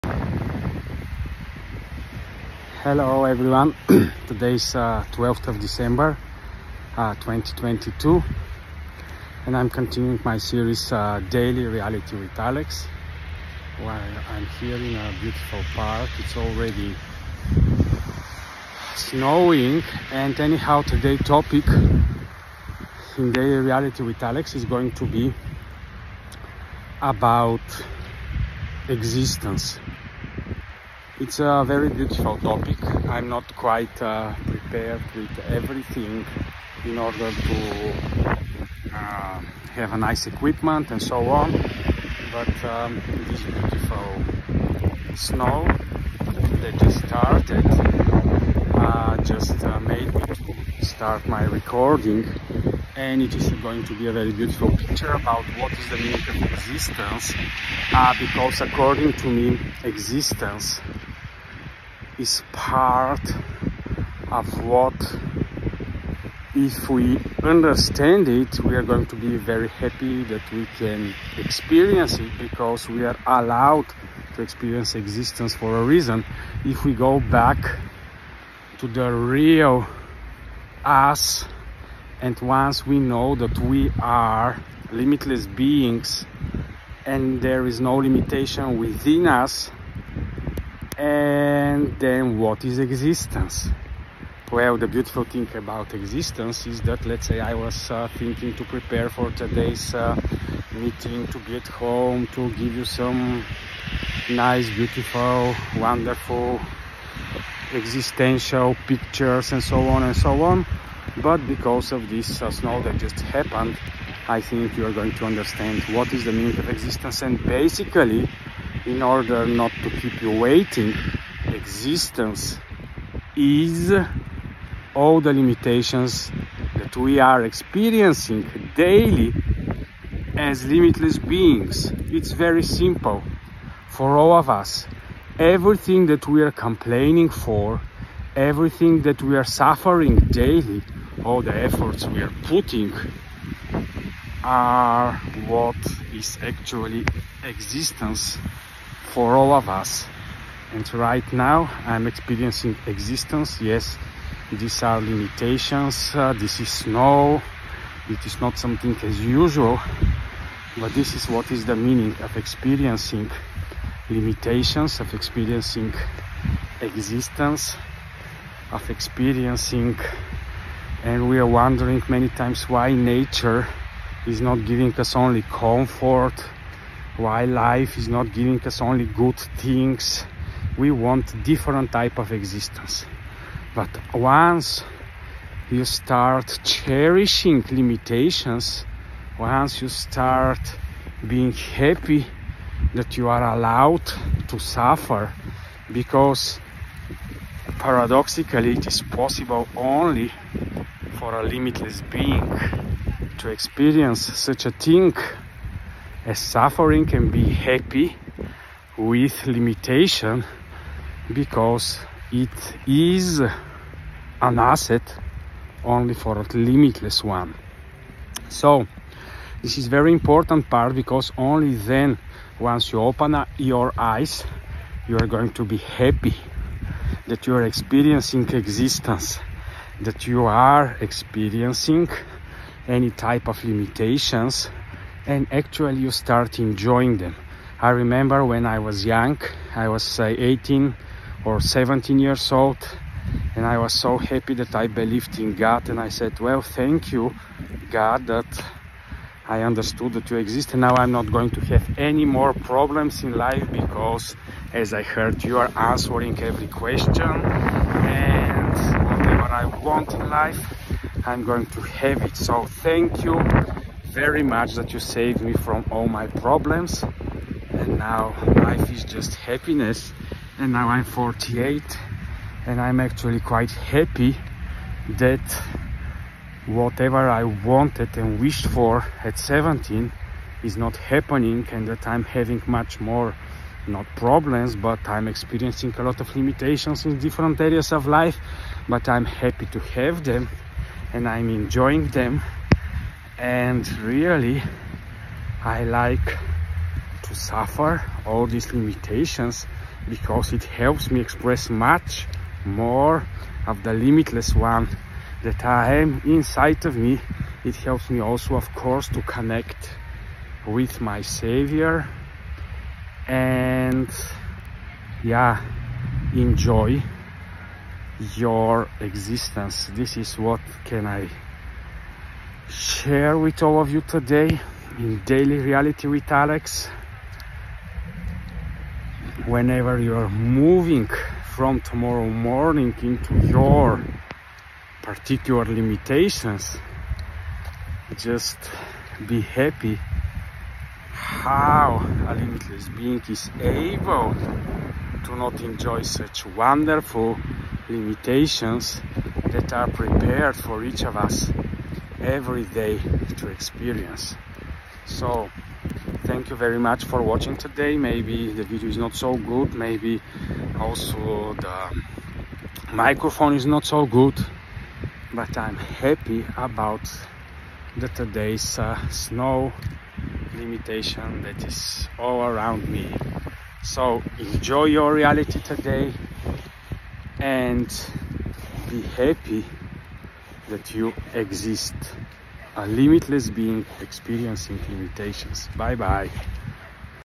Daily Reality with Alex 12.12.2022 What’s Existence
Well All of you my YouTube Reality Masters I am so glad 🙂 and happiness to share with you today the meaning of Existence

Yes It’s Fundamental and We never ask ourselves what is it

I would like so much to share it with all of you that I will let after watching the Video you live me a commitment with your thoughts on what I think or do you have a nicer and more empowering belief about your existence here in this Realm